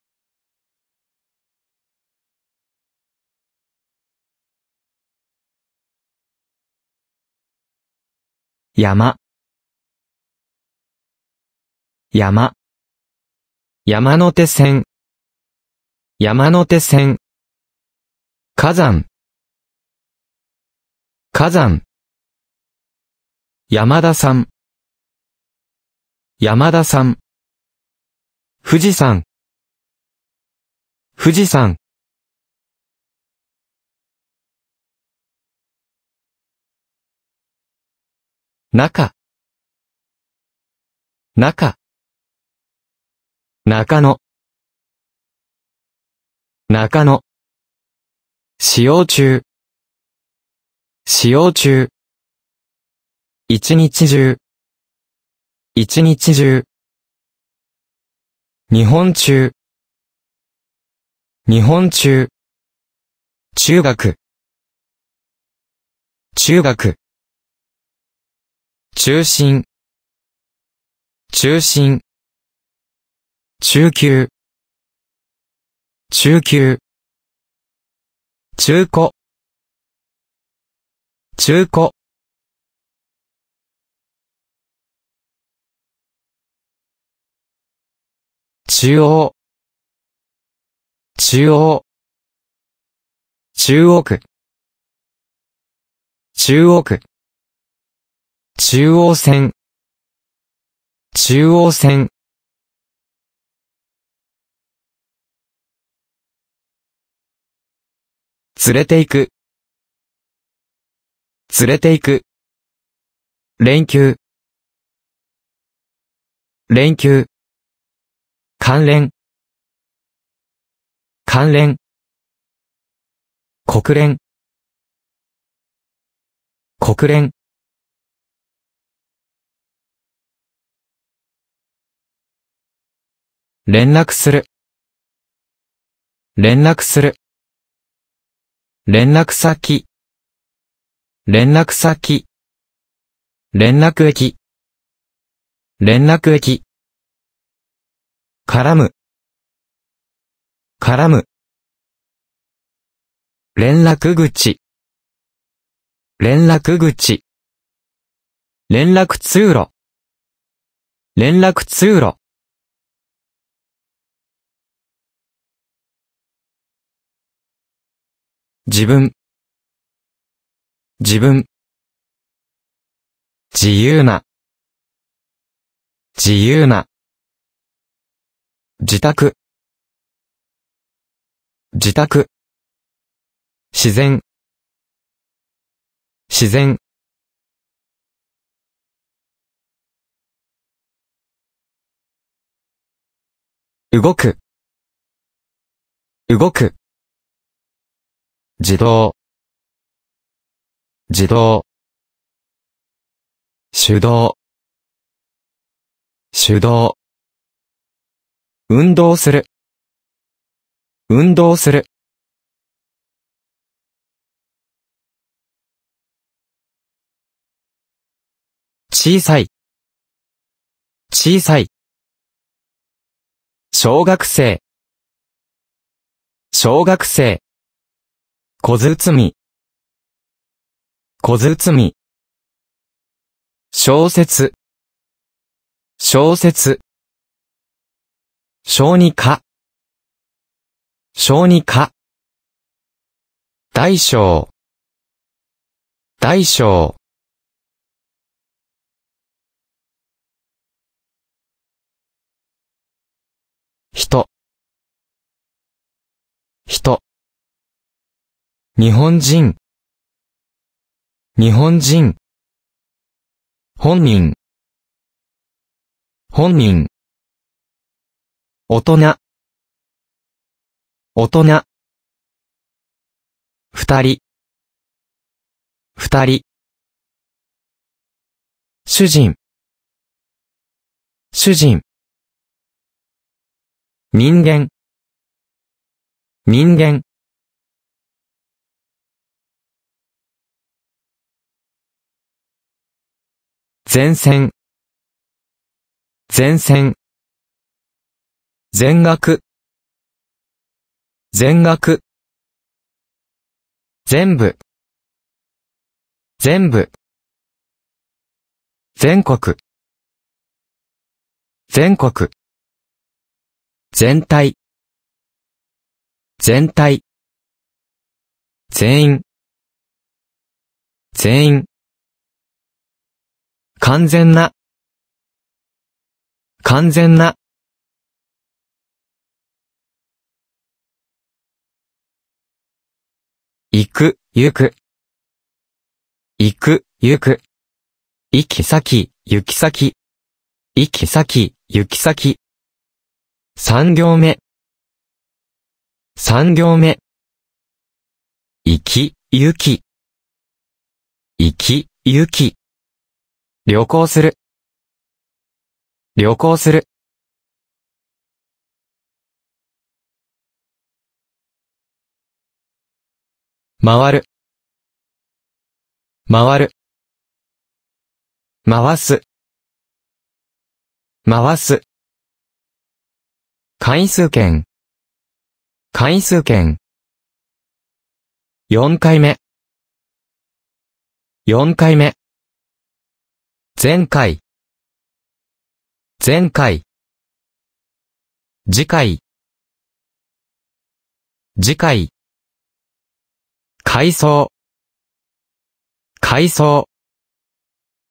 250 Essential Kanji for Everyday Use, Lesson 7 Vocabulary: How to Buy Train Tickets
The following Kanji characters are in the video: 山 中 央 連 絡 自 動 小 人 全 行 回